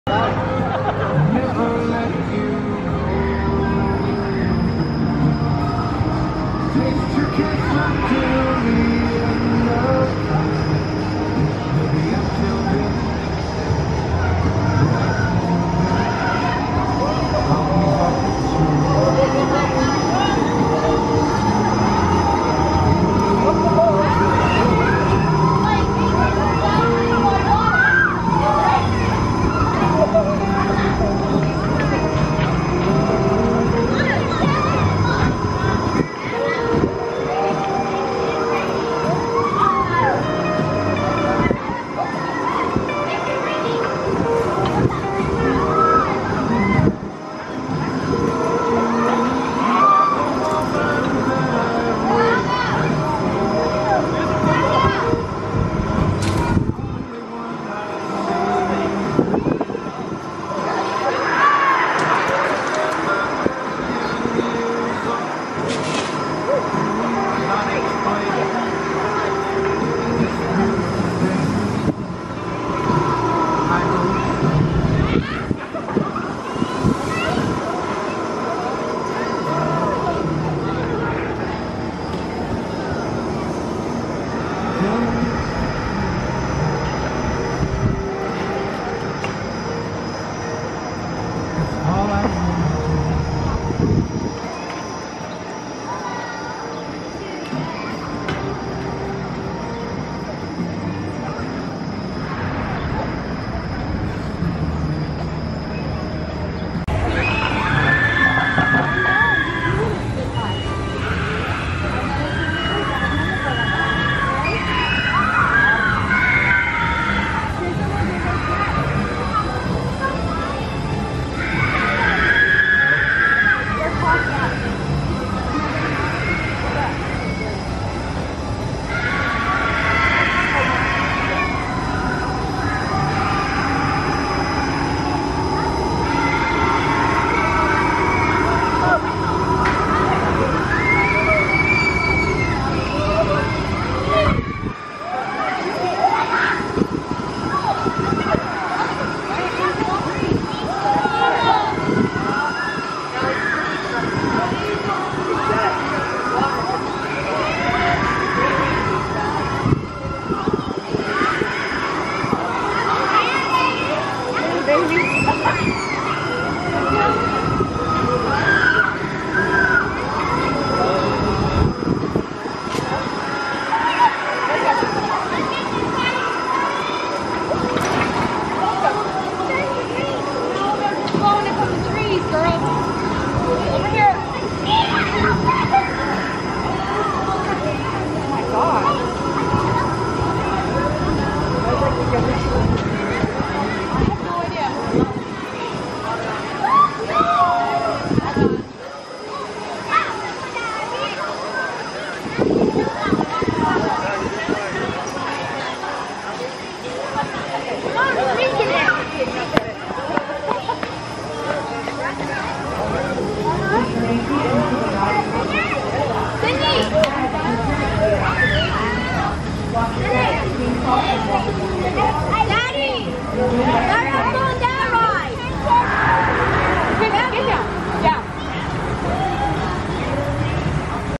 Frontier Flyers (snapping) Off Ride - Hersheypark - Copyright Free Footage
This footage is of the Larson Flying Scooters named Frontier Flyers at Hersheypark filmed in June 2024, featuring snapping. This footage is free to use in your video as long as you credit this channel/Coaster College in some way.